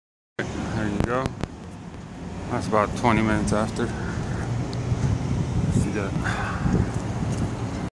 A ruined sundayscape...
Why ruined, just cause its above our heads don't mean we're safe. I have family members who get flu like symptoms, headaches, nausea & loss of appetite. Possibly even "fibro mialgia".